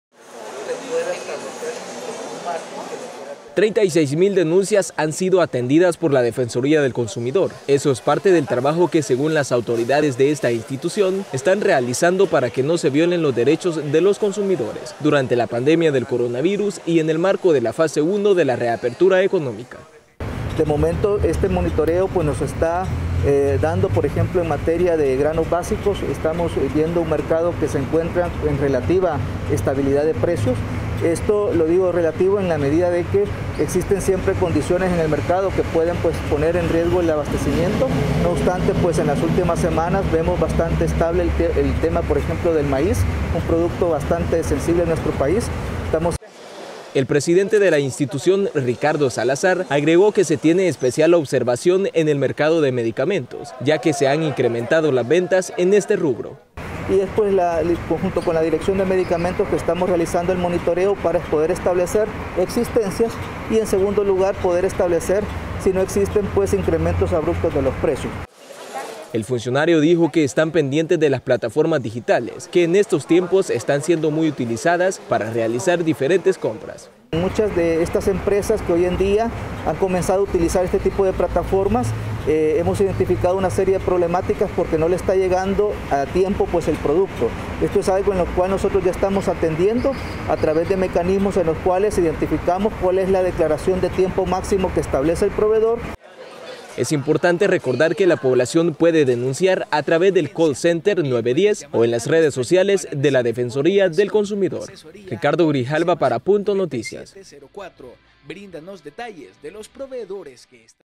Defensoría del Consumidor mantiene verificación de precios en los diferentes mercados de El Salvador
Con el fin de evitar abusos a la población la Defensoría del Consumidor se mantiene alerta del funcionamiento de los diferentes mercados, esto durante la pandemia del covid-19.

¡Visítenos!